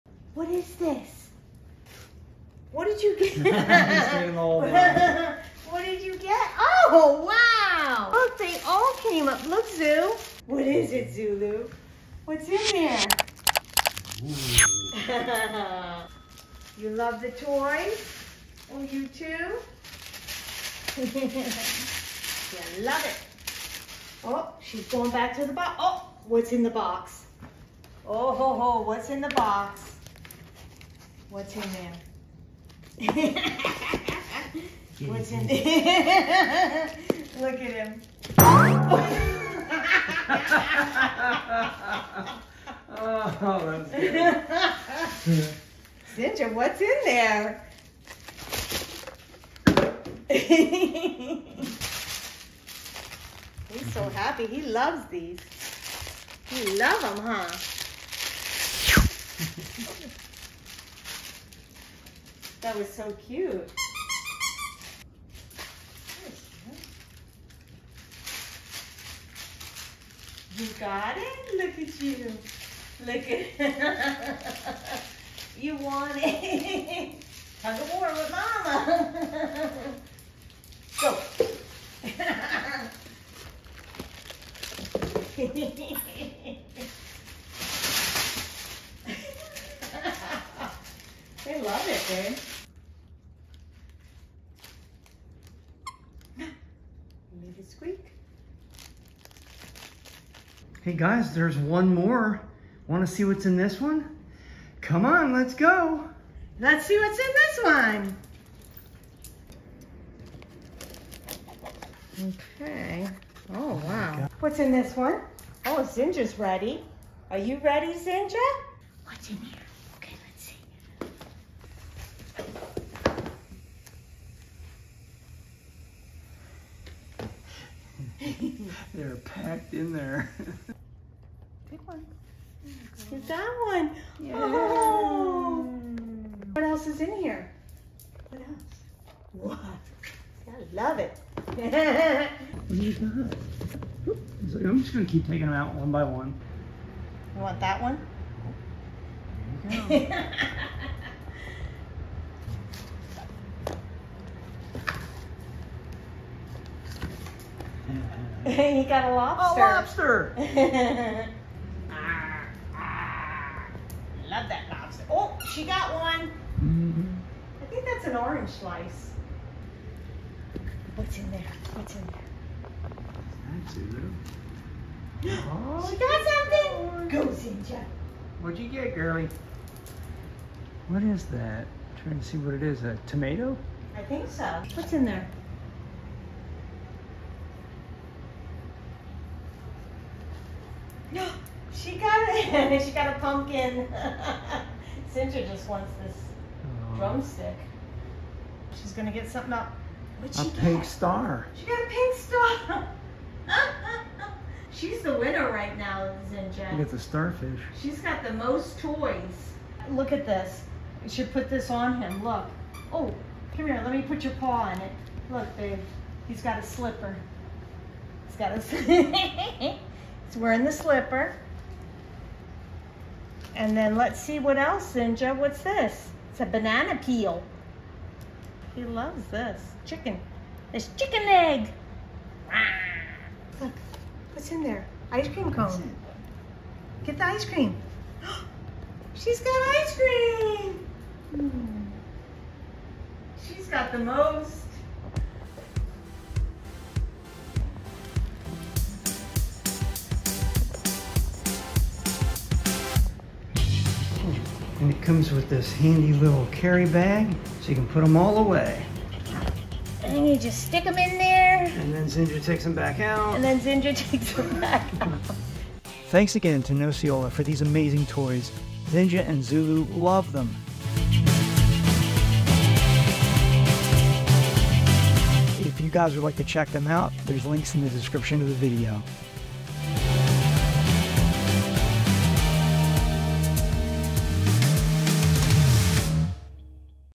We Got New Toys From nocciola !
We'd like to thank the sponsor of this video, Nocciola, for sending Zinga and Zulu two boxes stuffed full of toys!

🐾 Come Explore With Us! 🐾